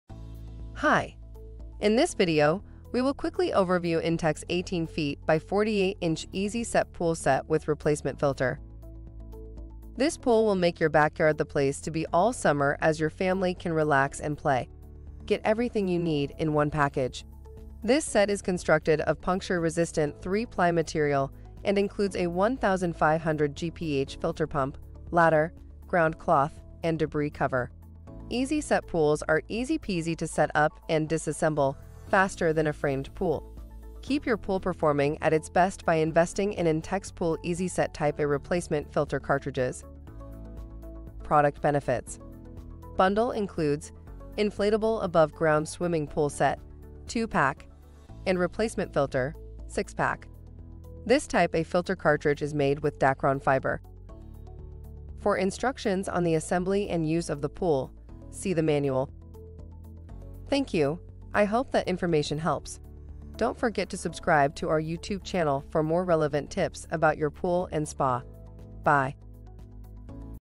INTEX 18ft x 48in Easy Set Pool Set with Replacement Filter
In this video, we will quickly overview INTEX 18ft x 48in Easy Set Pool Set with Replacement Filter

This pool will make your backyard the place to be all summer as your family can relax and play. Get everything you need in 1 package. This set is constructed of puncture resistant 3 ply material and includes a 1,500 GPH filter pump, ladder, ground cloth, and debris cover. Easy Set pools are easy-peasy to set up and disassemble faster than a framed pool. Keep your pool performing at its best by investing in Intex Pool Easy-Set Type A Replacement Filter Cartridges.

Product Benefits.
1) Bundle Includes: Inflatable Above Ground Swimming Pool Set (2 Pack) & Replacement Filter (6 Pack).
2) This Type A filter cartridge is made with Dacron fiber.

For instructions on the assembly and use of the pool, see the manual

USEFUL RESOURCES